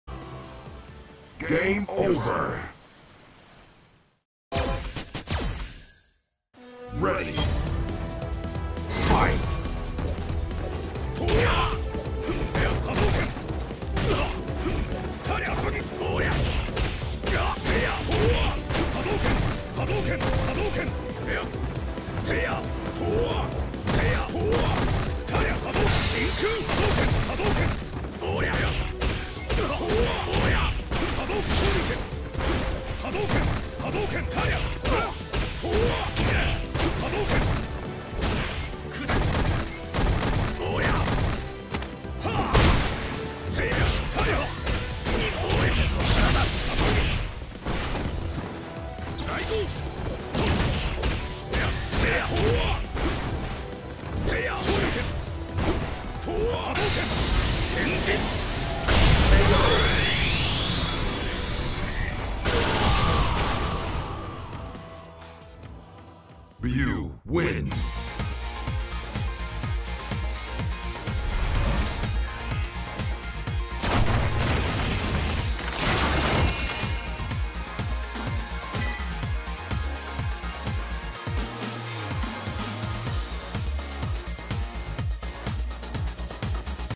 A True Martial Artist - CLUTCH MATCH! Manliest of Men Title Won! ^^
I have to admit that this was the most fiercest, dirtiest, strongest, and cheapest Ryu for Extra Battle Mode. This ate up my Fight Money a few times, but I finally got him during a clutch moment! You can probably tell that I'm a noob at this, but I won regardless. ;)